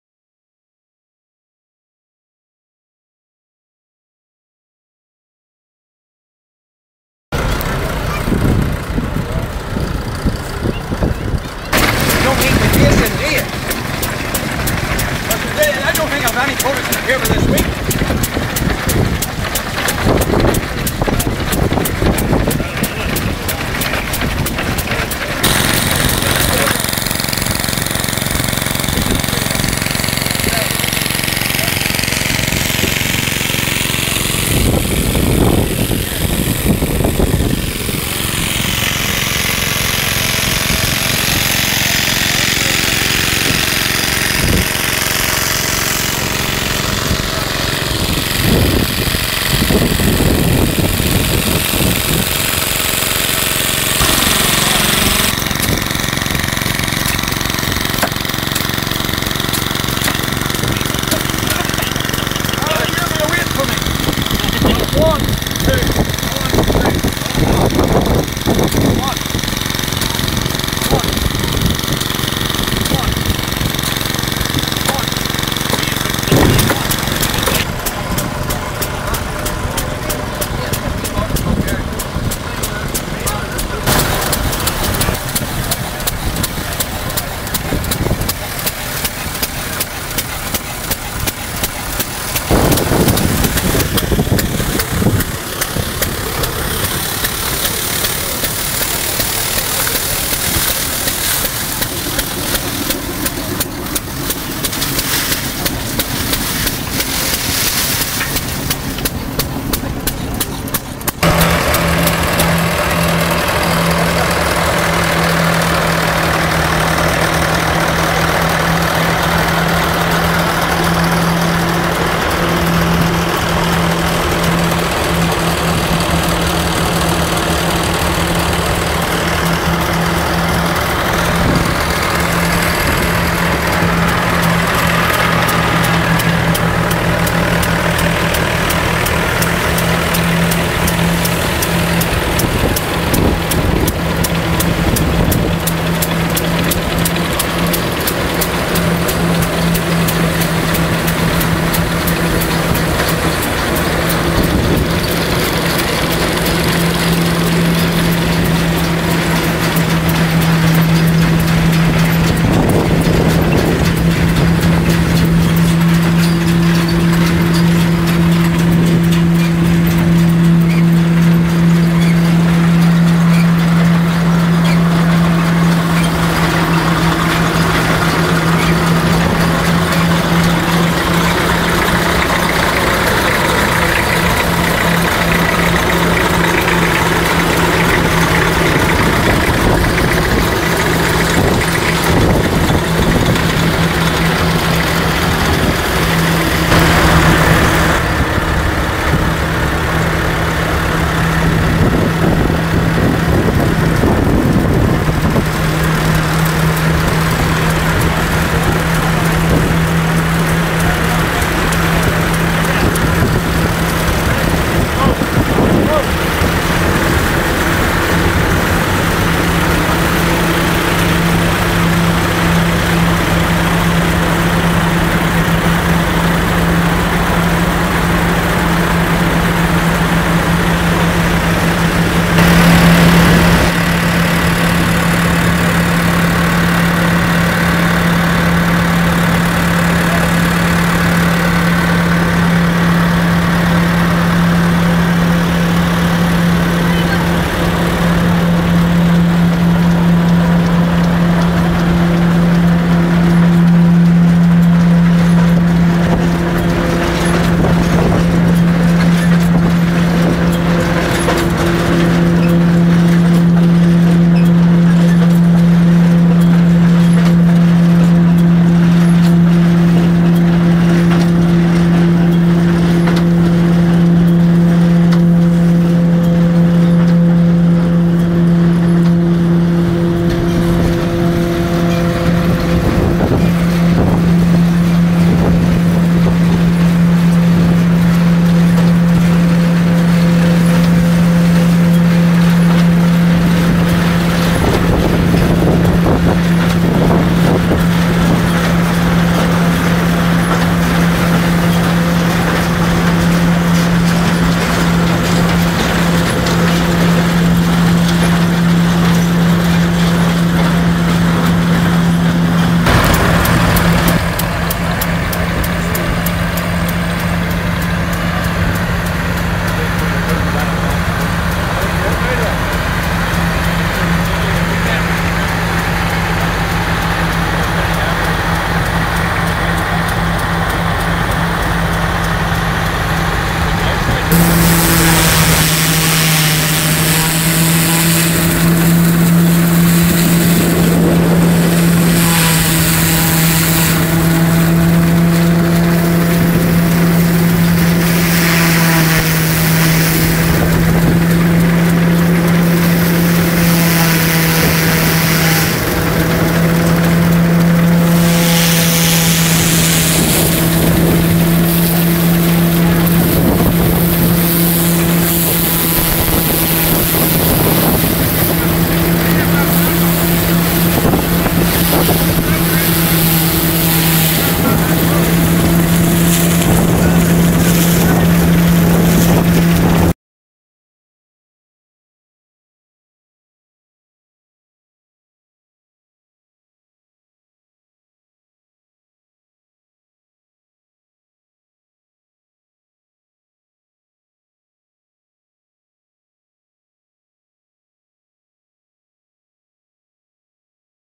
Vintage Threshing 2011, vol 1.wmv
Lower Mourne Vintage Club's annual display of old farm machinery, cars and methods of farming. This year £2700 was raised in aid of Mourne Stimulus Daycare Centre.